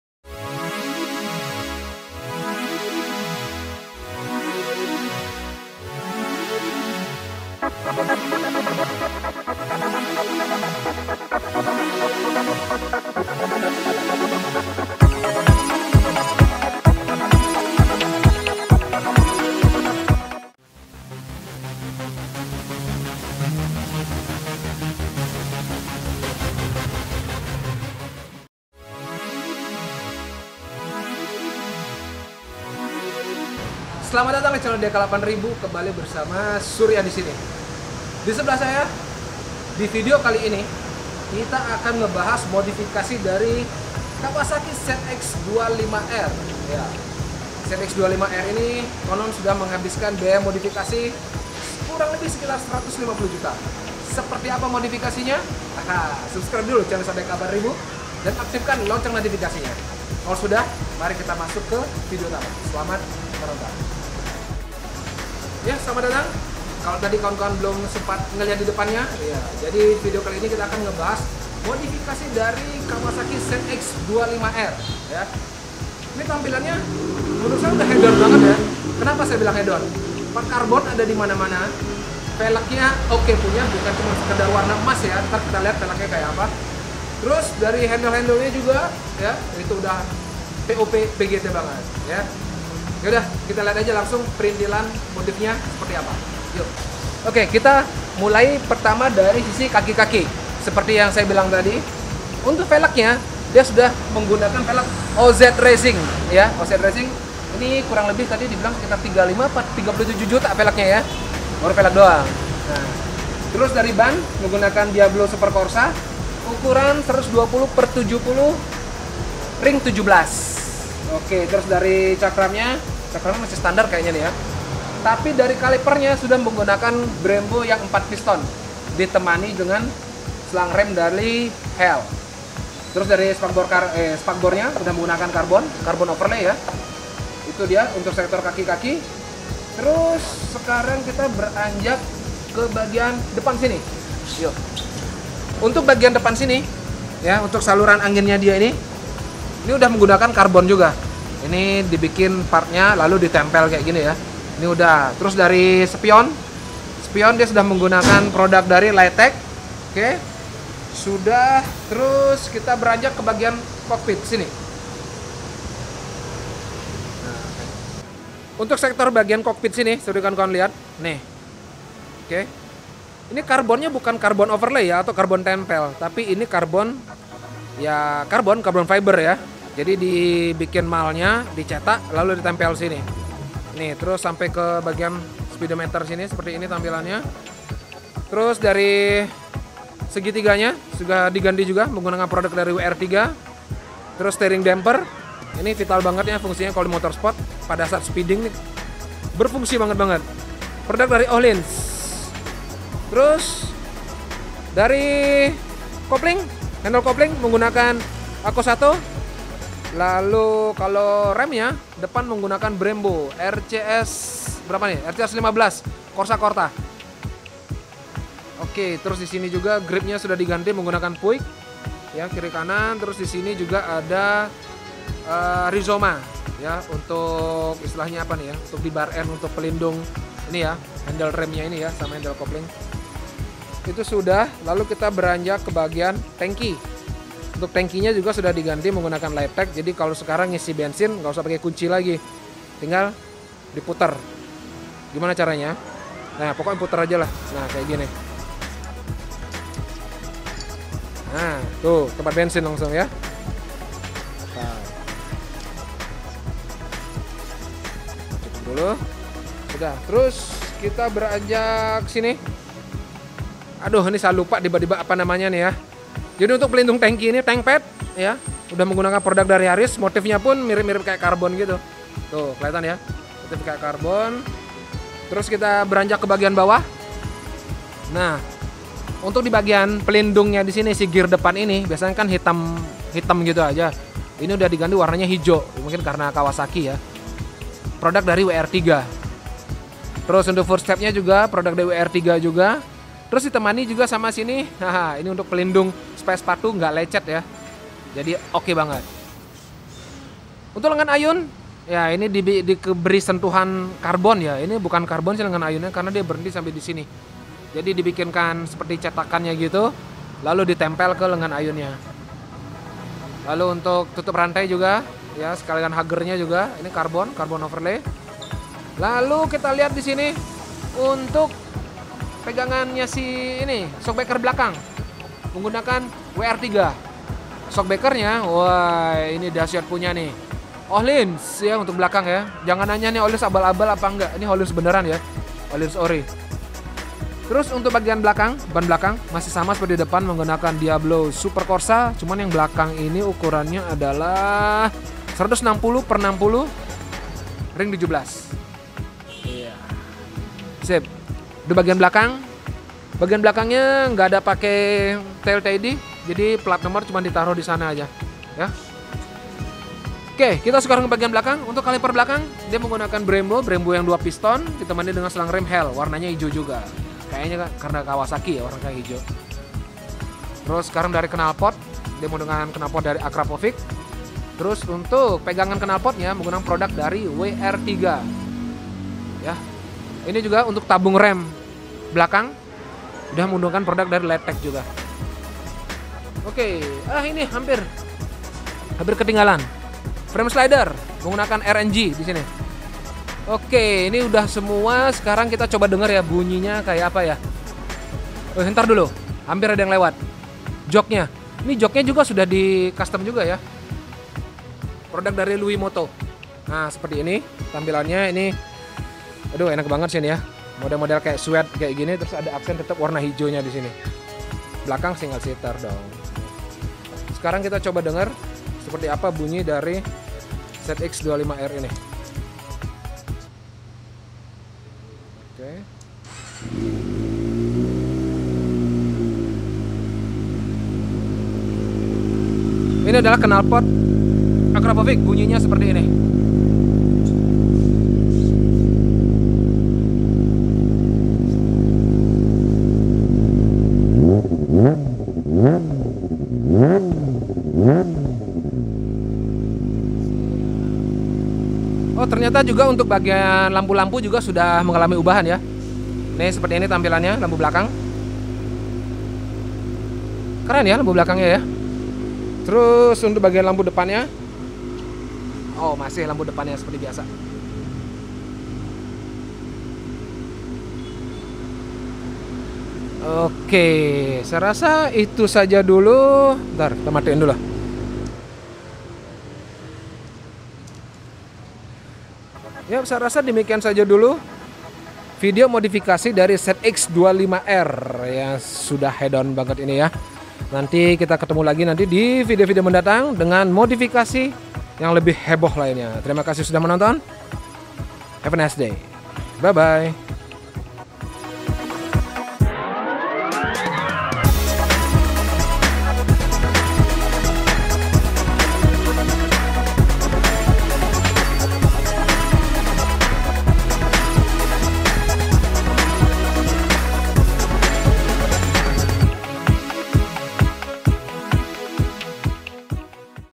Modifikasi Kawasaki ZX25R | Velg nya cuma 35 Juta
Modifikasi Kawasaki ZX25R yang ada pada video ini keren abis. Mungkin kawan-kawan kalau melihat sekilas terlihat modifnya biasa saja. Namun jika diperhatikan lbih detail, Modifikasi Kawasaki ZX25R sudah hedon. Total biaya yang sudah dihabiskan untuk Modifikasi Kawasaki ZX25R ini seitar 150 juta rupiah. Wow.. Apa saja part-part nya? Silakan tonton videonya sampai habis ya